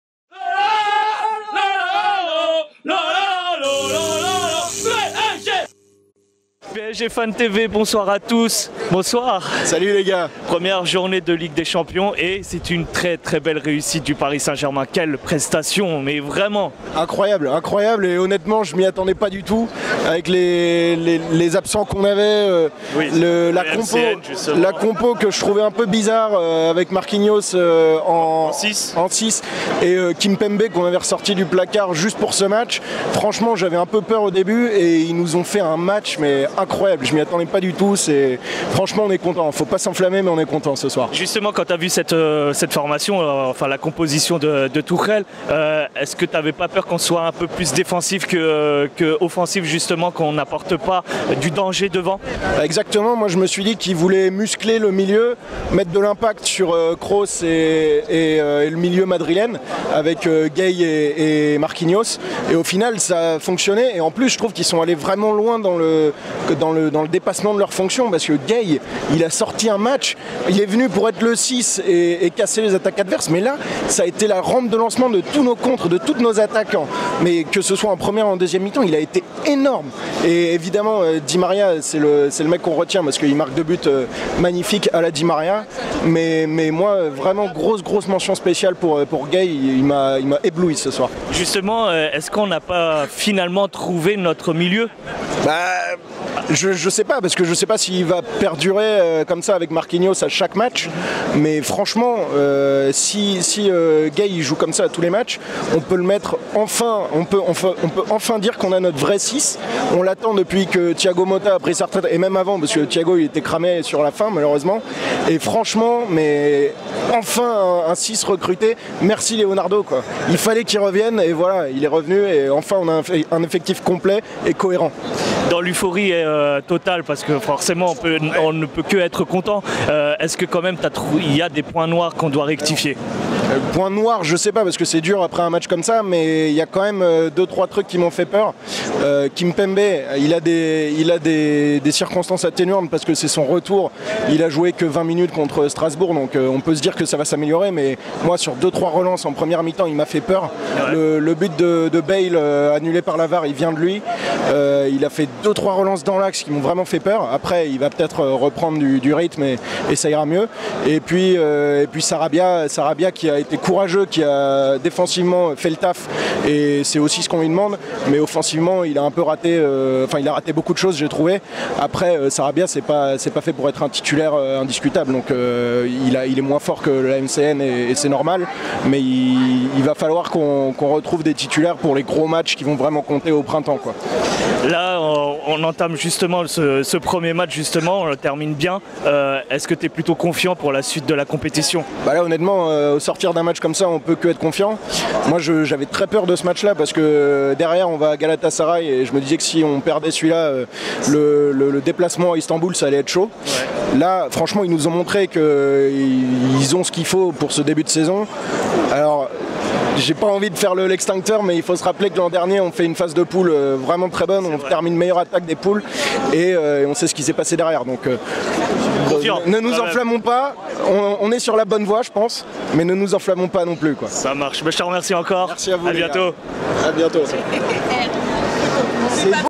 PSG vs Real Madrid 3-0 | Enfin, On A Un Effectif Au Complet Et Cohérent ! (Antoine)
Les supporters parisiens analysent la rencontre du Paris Saint-Germain face au Real Madrid, lors de la 1ère journée de Ligue des Champions 2019/20. Les champions de France ont livrés une copie parfaite avec une énorme performance de Di Maria qui inscrit un doublé, et un Idrissa Gueye, omniprésent dans la récupération et la distribution.

C'est Parti !!